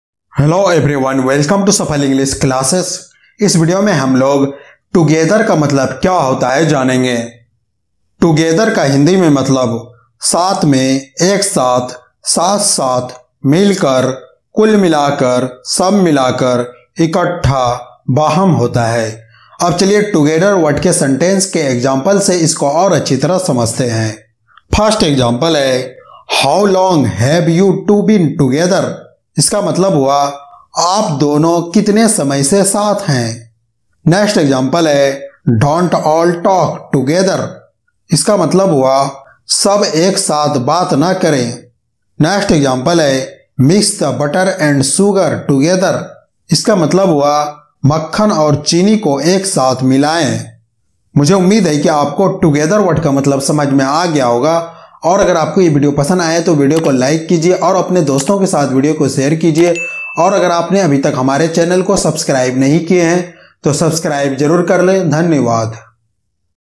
Together Ka Matlab Hindi Me | Together Meaning in Hindi | Together ka Matlab Kya hota hai
Aaj ki Is Video Me Maine Aapko Together Ka Matlab Kya hota hai Hindi mein or Together Word Ke Sentence ka Example ko batlaya hai.